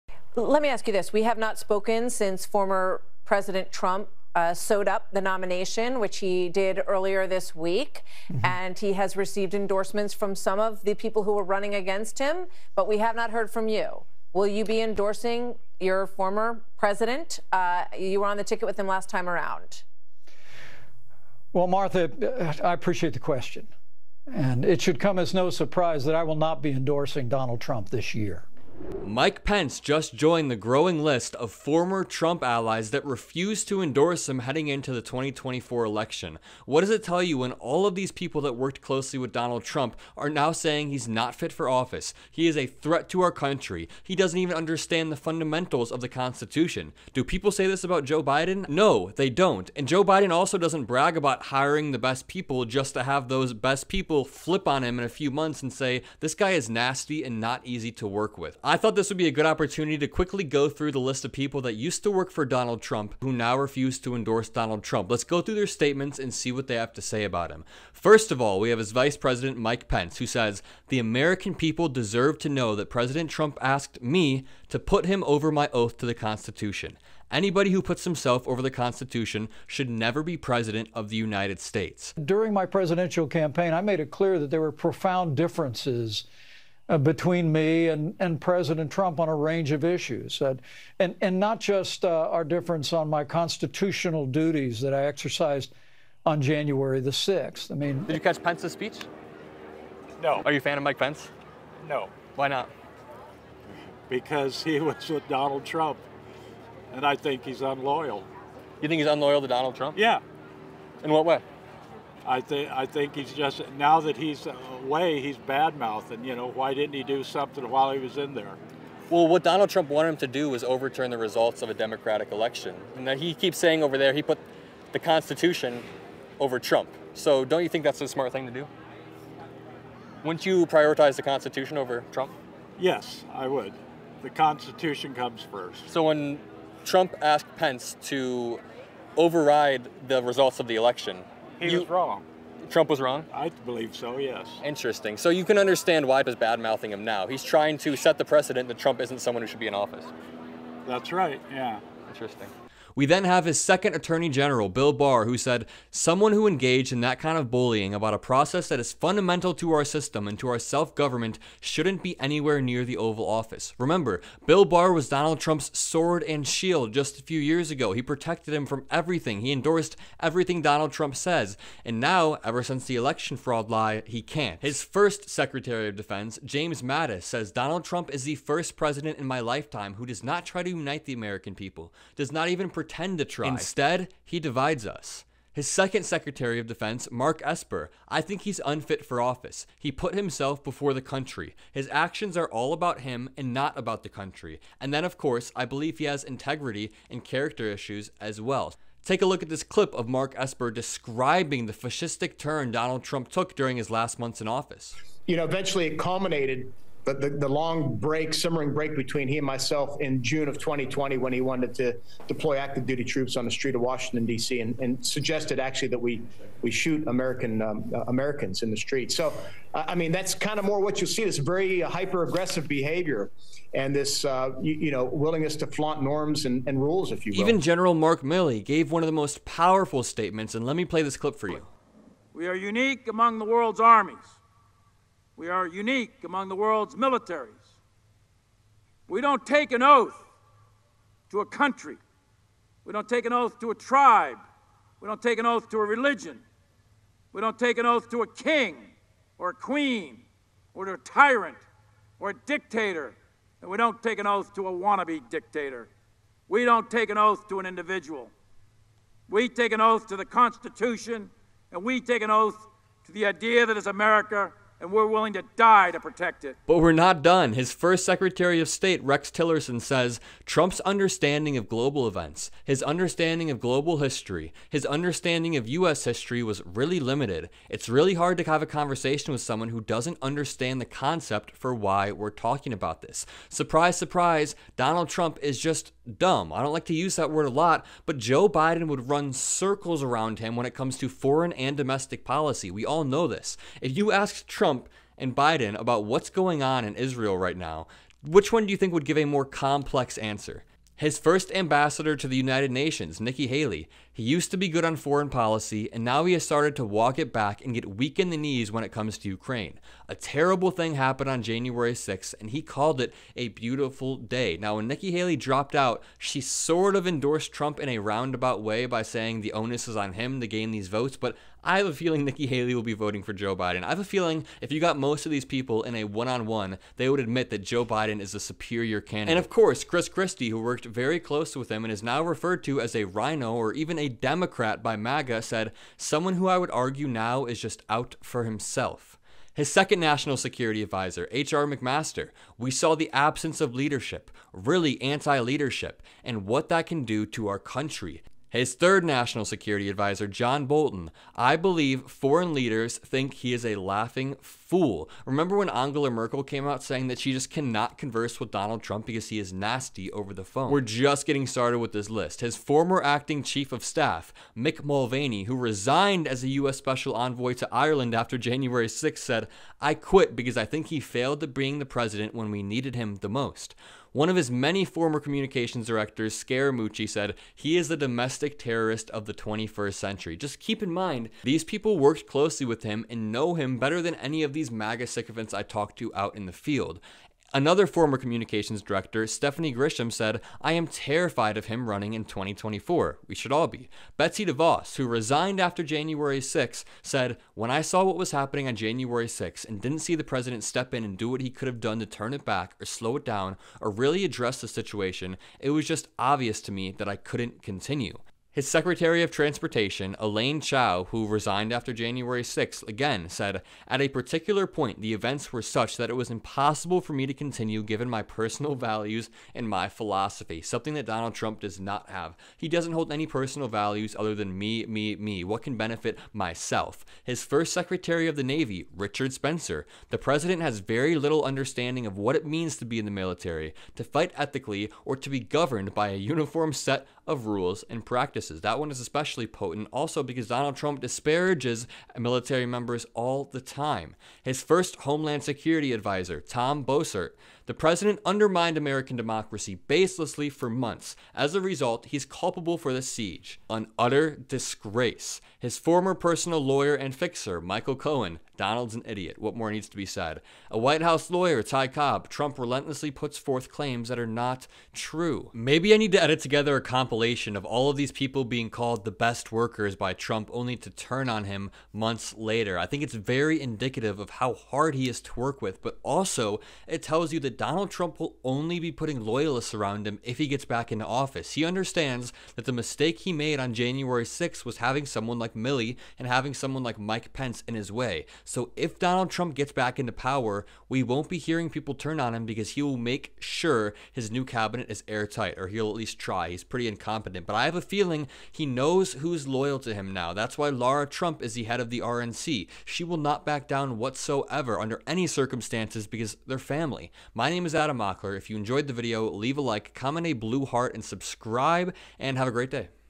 Trump’s Top Allies SUDDENLY TURN AGAINST HIM…
Mike Pence joins the long list of former Trump aids and allies who refuse to work with him again. Adam Mockler x MeidasTouch.

Adam Mockler - amock